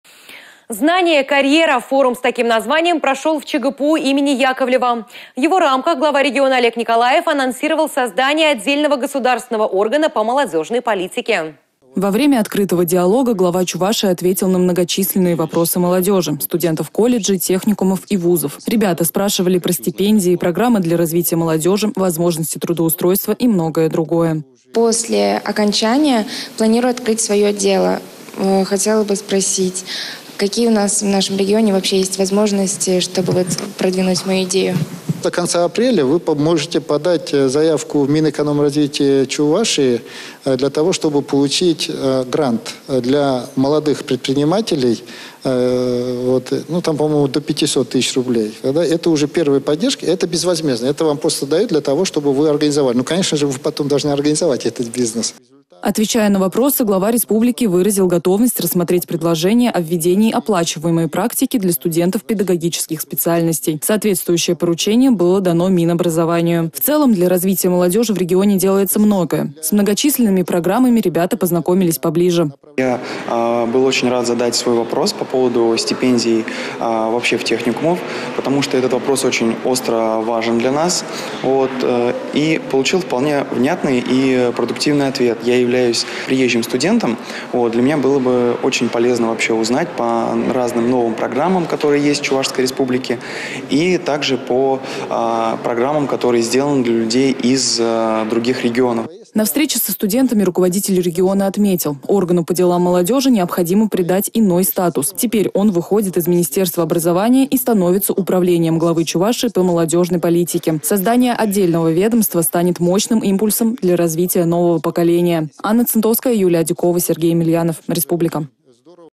В Чебоксарах прошёл молодёжный форум «Знание. Карьера»
«Знание. Карьера» – форум с таким названием прошел в ЧГПУ имени Яковлева. В его рамках Глава региона Олег Николаев анонсировал создание отдельного государственного органа по молодёжной политике.
Во время открытого диалога Глава Чувашии ответил на многочисленные вопросы молодёжи – студентов колледжей, техникумов и ВУЗов. Ребята спрашивали про стипендии, программы для развития молодёжи, возможности трудоустройства и многое другое.
– После окончания планирую открыть своё дело. Хотела бы спросить, какие у нас есть возможности, чтобы продвинуть своё дело?
– До конца апреля вы можете подать заявку в Минэк Чувашии, чтобы получить грант для молодых предпринимателей. Там до 500 000 рублей, которые безвозмездно дают для организации, но конечно вы должны организовать этот бизнес. Отвечая на вопросы, Глава республики выразил готовность рассмотреть предложение о введении оплачиваемой практики для студентов педагогических специальностей. Соответствующее поручение было дано Минобразованию. В целом, для развития молодёжи в регионе делается многое – с многочисленными программами ребята познакомились поближе.
ЛЕОНИД ТРОФИМОВ: «Я был очень рад задать вопрос по поводу стипендии в техникумах. Этот вопрос очень важен для нас. Получил внятный и продуктивный ответ. Я являюсь приезжим студентом, для меня было очень полезно узнать про программы, которые есть в Чувашии и про программы, которые сделаны для людей из других регионов».
На встрече со студентами руководитель региона отметил – органу по делам молодёжи необходимо придать иной статус. Теперь он выходит из министерства образования и становится Управлением Главы Чувашии по молодёжной политике. Создание отдельного ведомства станет мощным импульсом для развития нового поколения.